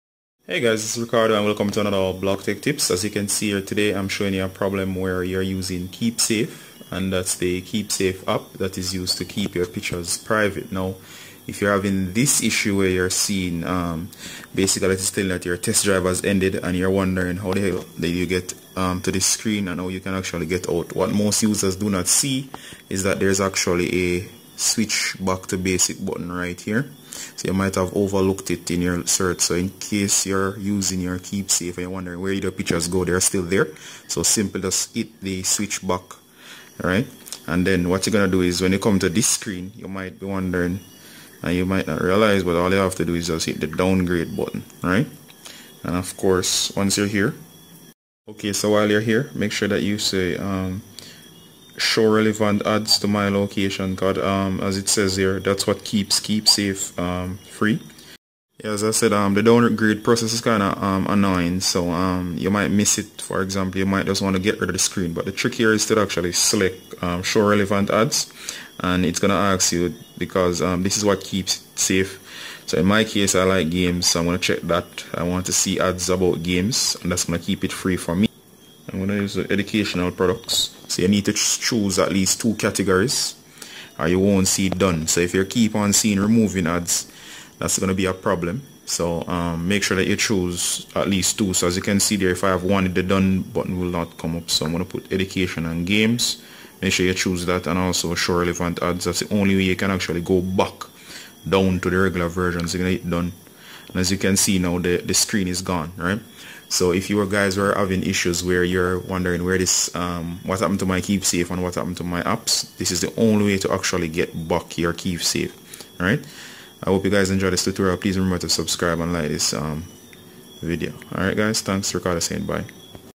Keepsafe Your Test drive has ended Fix
If you are seeing the Your Test drive has ended screen in Keepsafe this is how you get rid of it and regain access to your files easy.